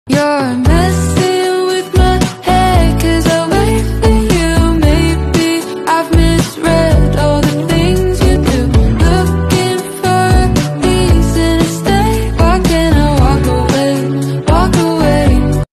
Careful final checking and record before delivery
All 50 vacuum circuit breakers sent to Saudi Arabia are ready to be dispatched! Providing whole set of VCB parts & switchgear accessories,
RMU as well as accessories.